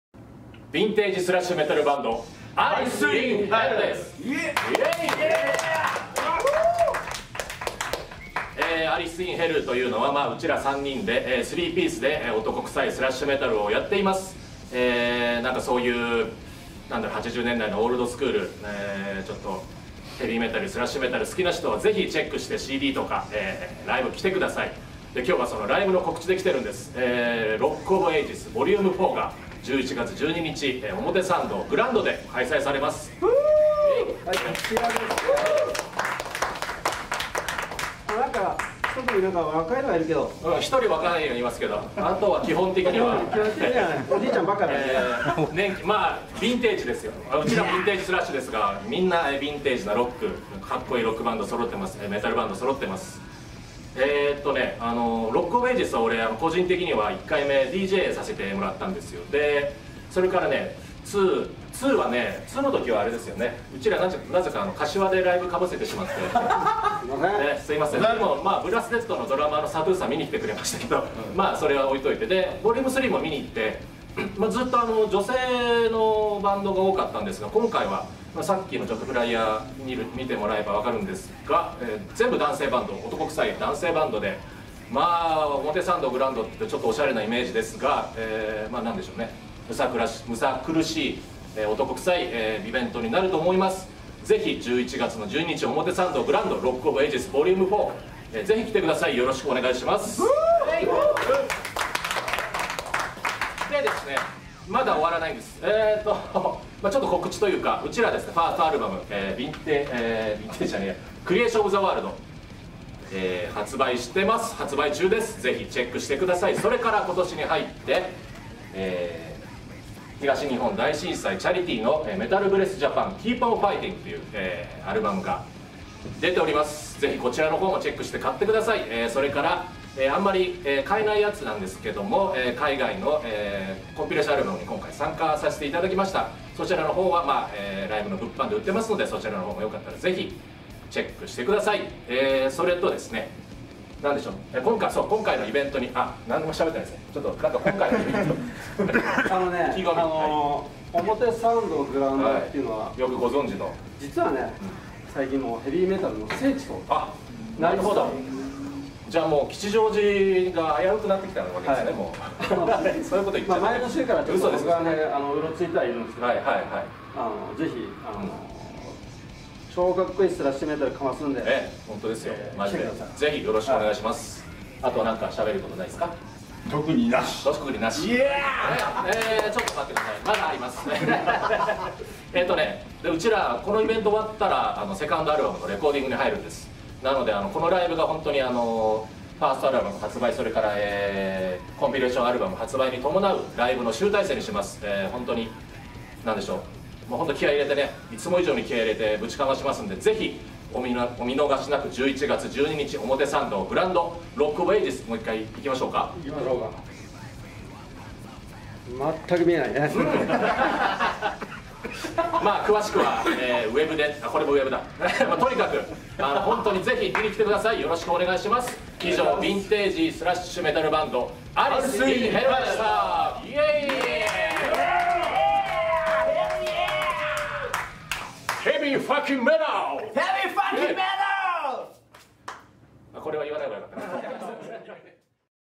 ALICE IN HELL からコメント
2016年11月12日（土）東京：表参道GROUNDで開催される国内インディー・メタル・ライヴ・イベント『ROCK of AGES Vol.4』に出演する、ヴィンテージ・スラッシュメタル・バンド ALICE IN HELL の皆さんからコメント。

リーダーの浜手 "HAMADIE" 大典氏（Vo&Gt)によるコラム